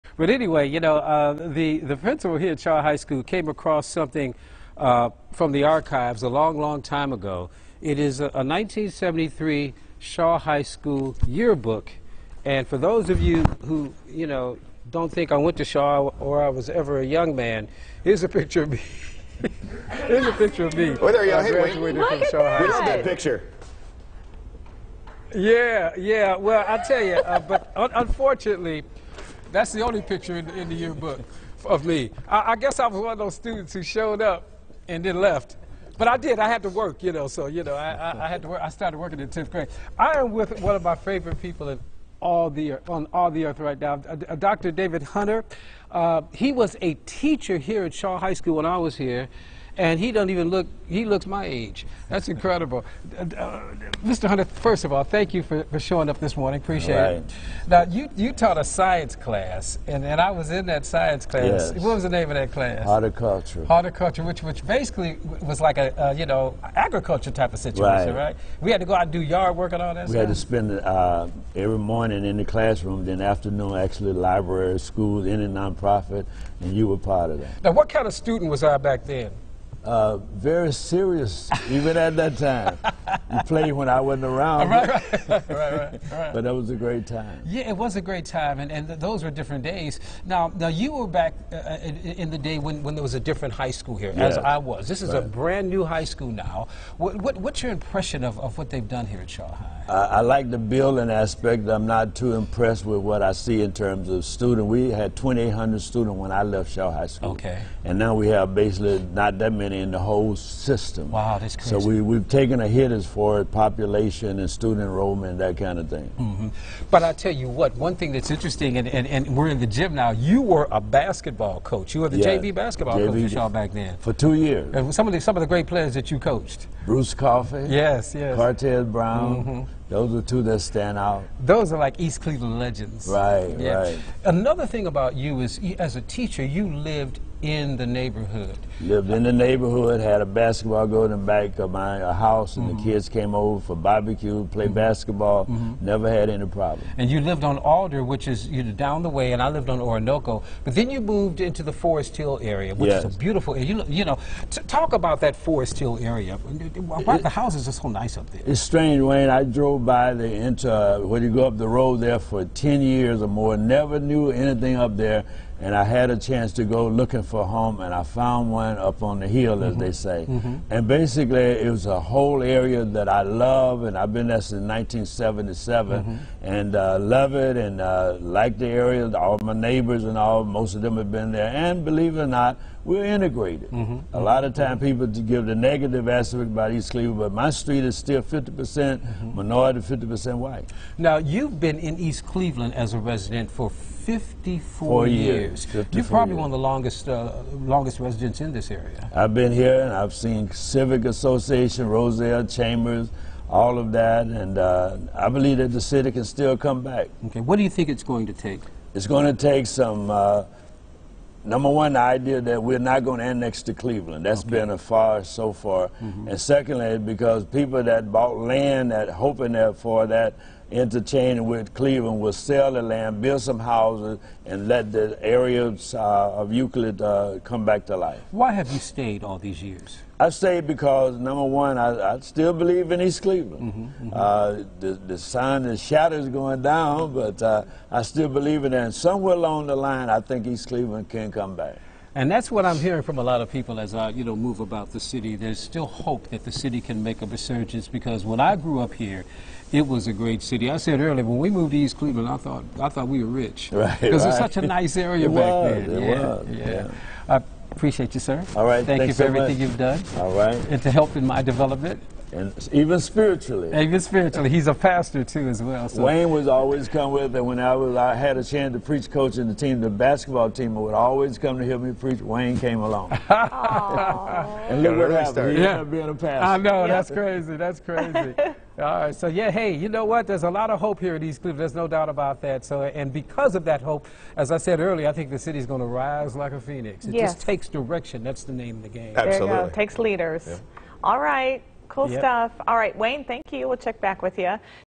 Hometown Anchor: Wayne Dawson
Fox 8's Wayne Dawson has fond memories of his high school days at Shaw High School in East Cleveland. He caught up with one of his former  teachers to re-live some of his days in science class.